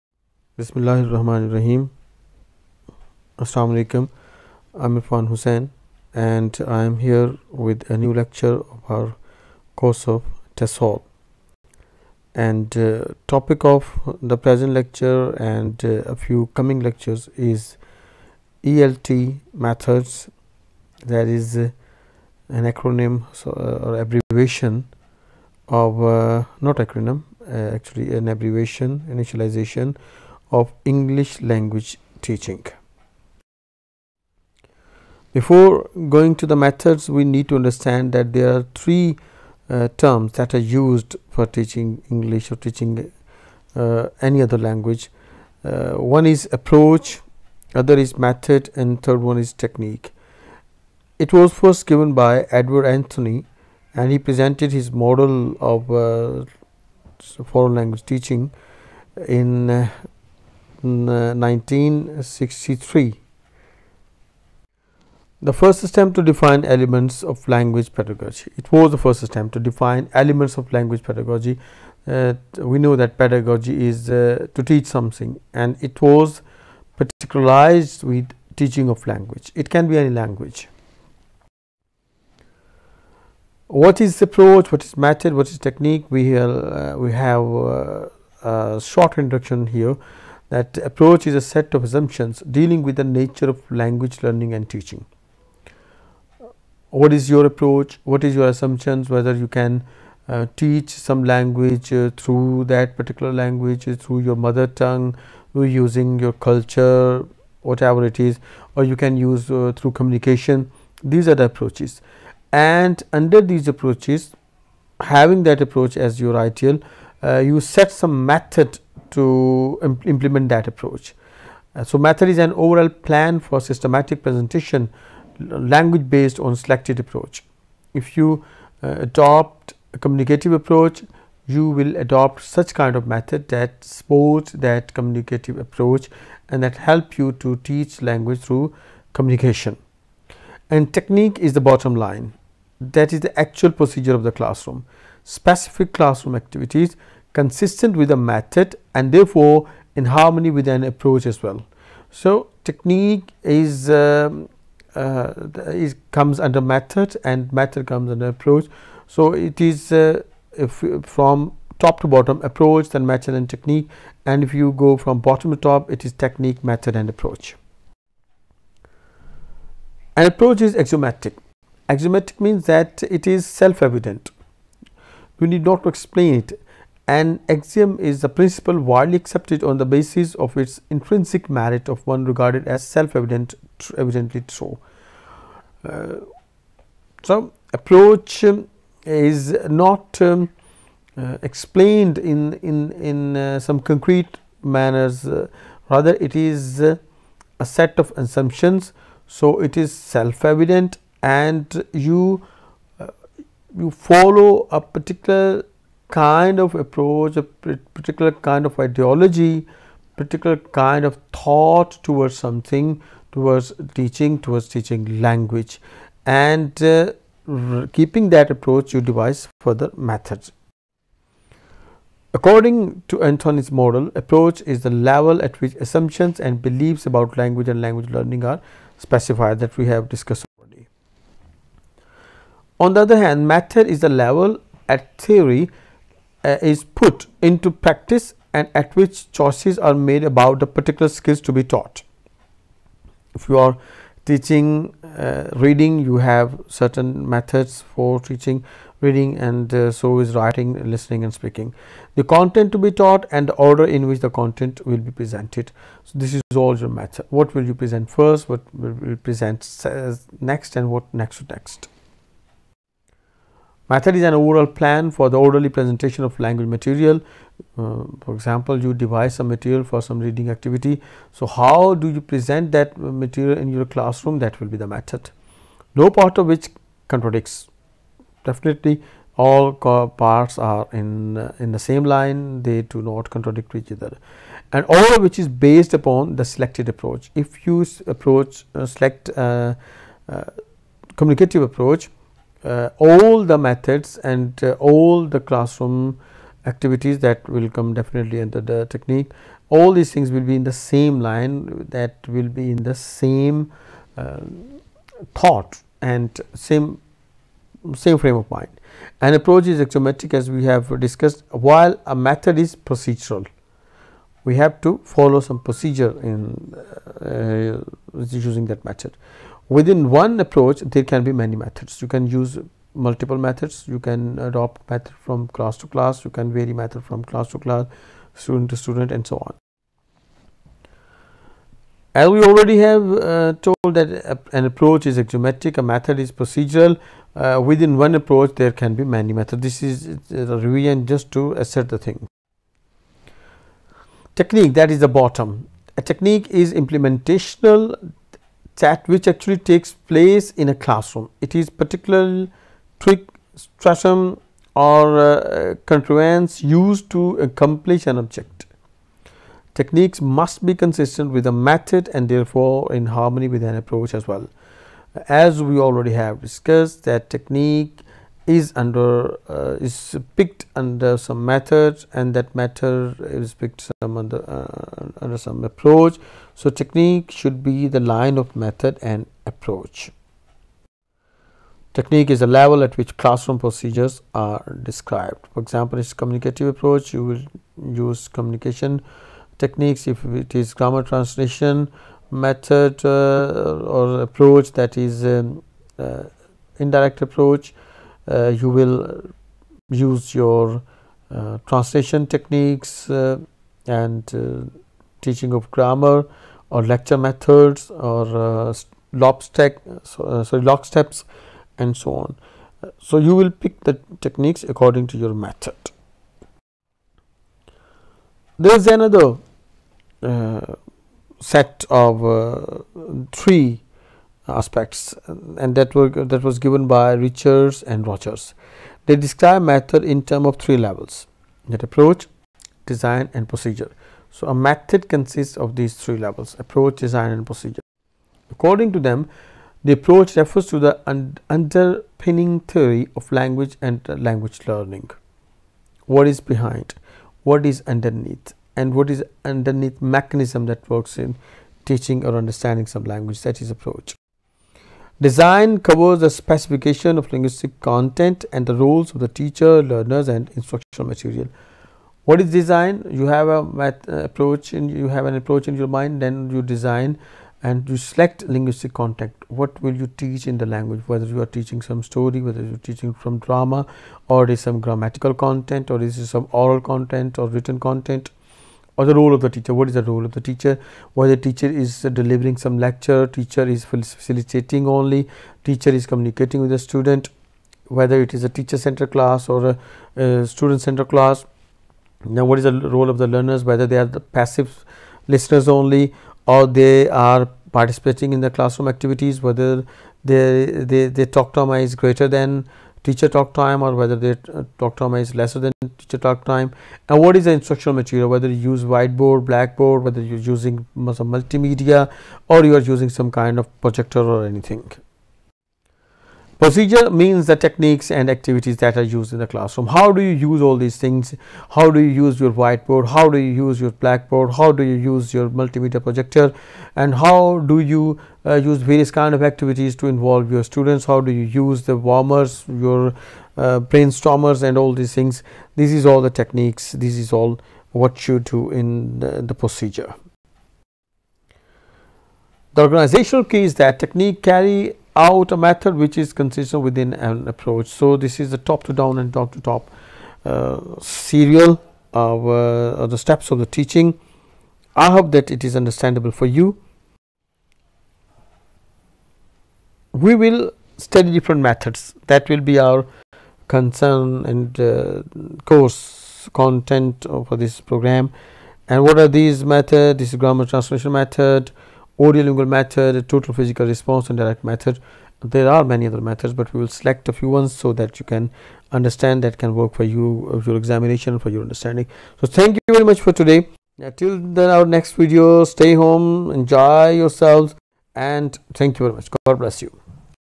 English Language Teaching Method
This video is a brief explanation of the difference between Approach, Method and Technique.